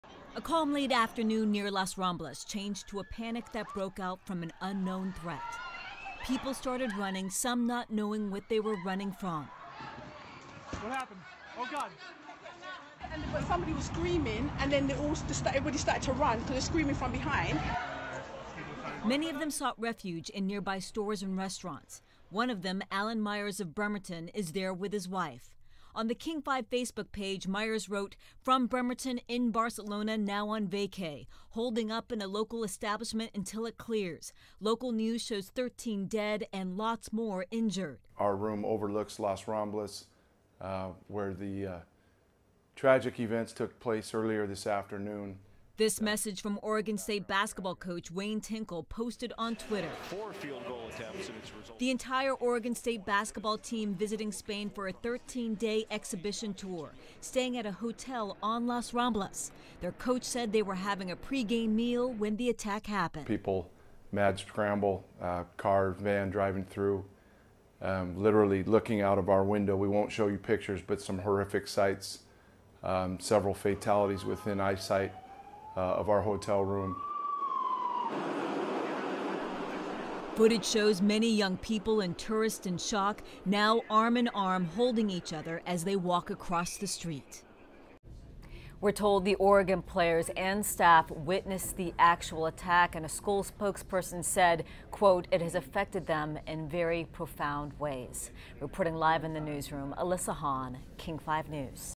Pacific NW tourists in Barcelona during attack
KING 5's Elisa Hahn reports.